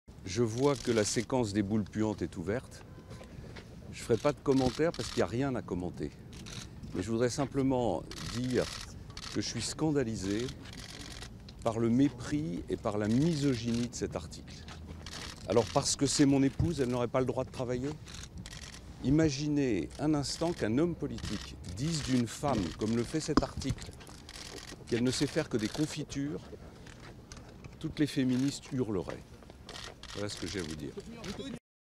Fillon "scandalisé" par les accusations du Canard Enchaîné
Alors que le Canard Enchaîné accuse la femme du candidat des Républicains à l'élection présidentielle d'emploi fictif en tant qu'attaché parlementaire de son mari, François Fillon a qualifié cette accusation mercredi de "boule puante" lors de la visite du site de Thalès en compagnie d'Alain Juppé. SONORE